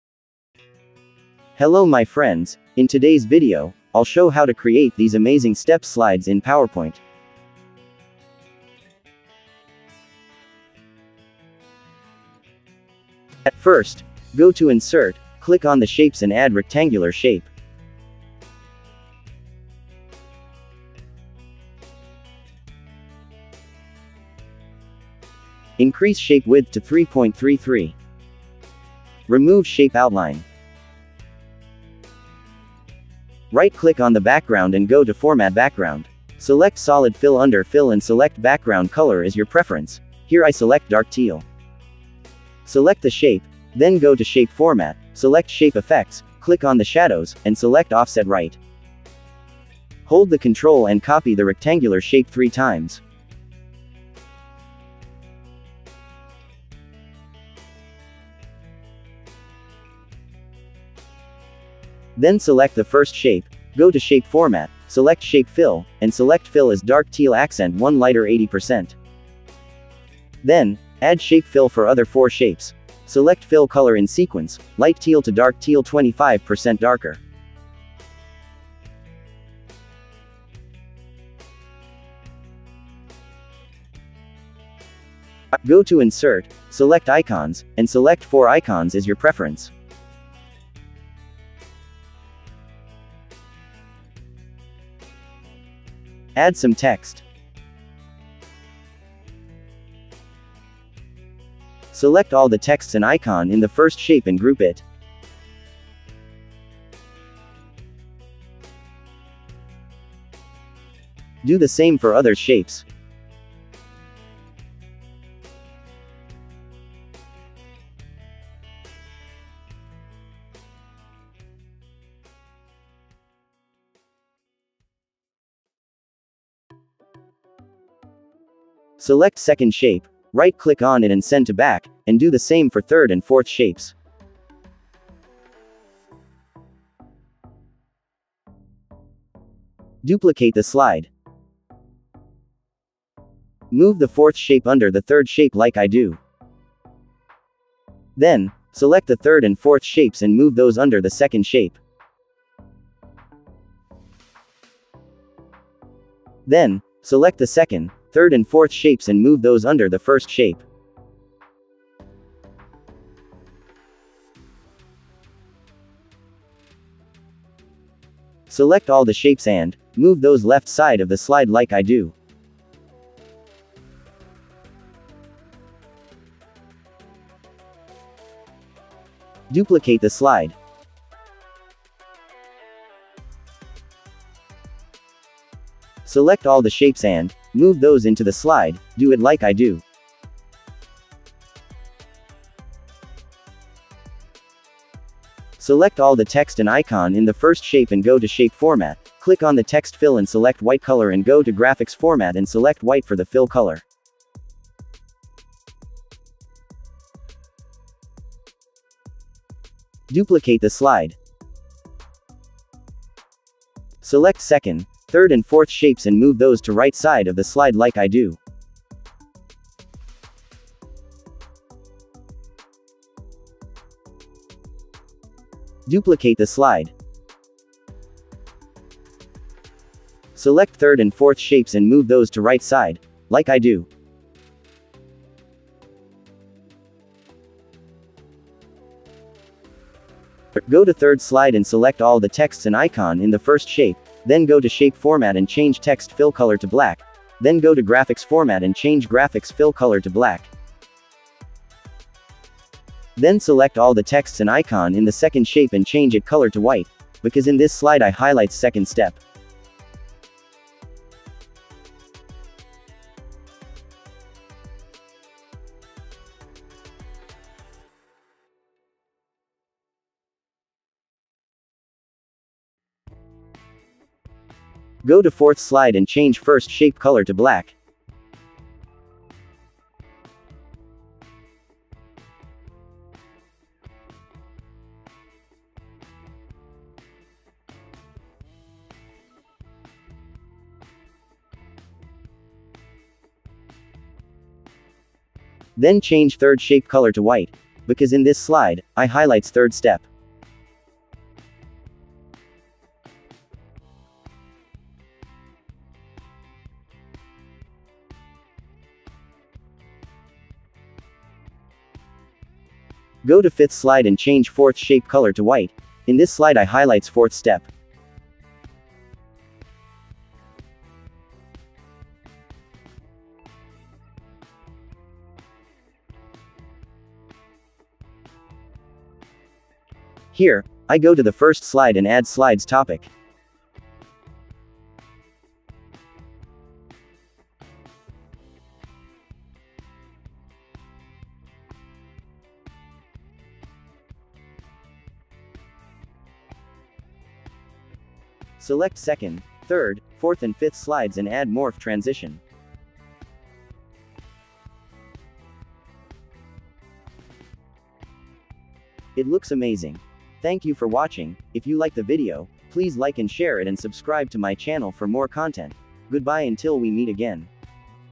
Animated Step Slides Tutorial in PowerPoint
In this comprehensive tutorial, learn how to create dynamic and visually appealing animated step slides in PowerPoint. Whether you're a beginner or an experienced user, this step-by-step guide will walk you through the process of designing captivating step-by-step slides that will keep your audience engaged and informed. From adding animations to incorporating transitions, you'll discover various techniques to make your presentations stand out. Follow along and unleash your creativity with PowerPoint's powerful features.